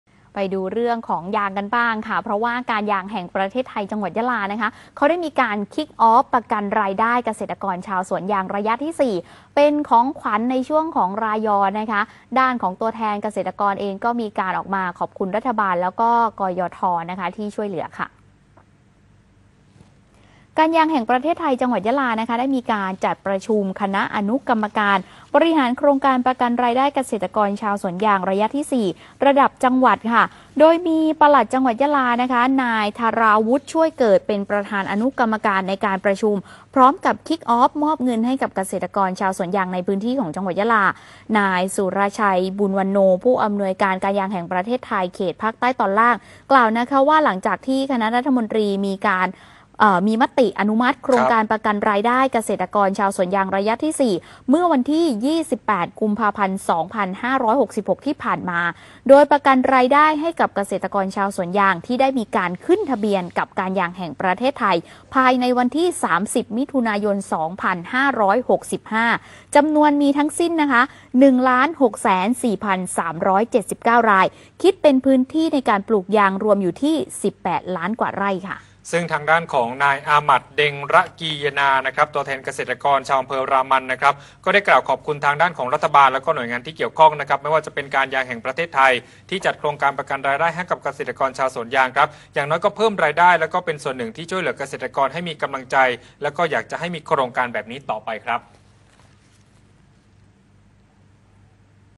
ข่าวเด่นประเด็นใต้ 13 เม.ย. 66 - กยท.จ.ยะลา Kick off ประกันรายได้เกษตรกรชาวสวนยาง ระยะที่ 4
ข่าวเด่นประเด็นใต้ ประจำวันที่ 13 เม.ย. 66 - กยท.จ.ยะลา Kick off  ประกันรายได้เกษตรกรชาวสวนยาง ระยะที่  4